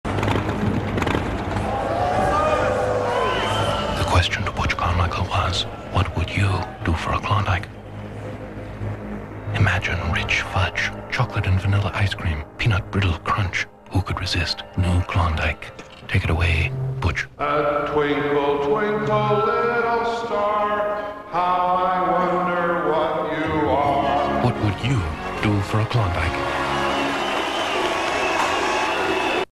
Klondike commercial from 1999
Pulled from VHS Tape of Sept 17th 1999 broadcast of Sex and the City and Repulsion on Bravo and August 22nd 1999 broadcast of Cocoon on A-Channel